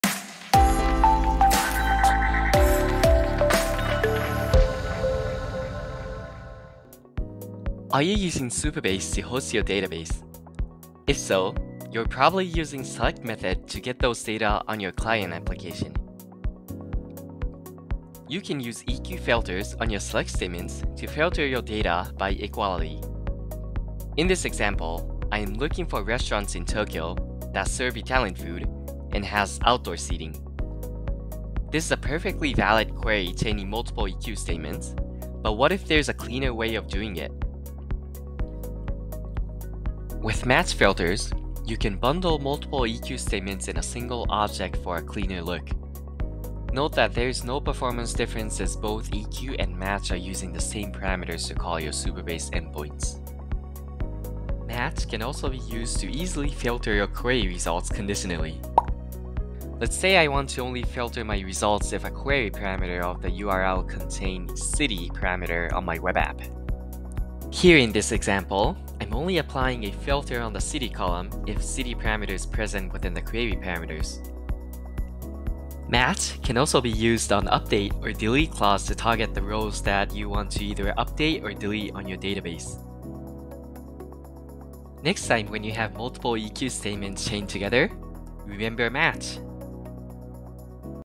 match - SupabaseTips
In Supabase, match filter allows you to bundle multiple eq filters in a single object. Learn how you can replace a single match filter with multiple eq filters to simplify your code.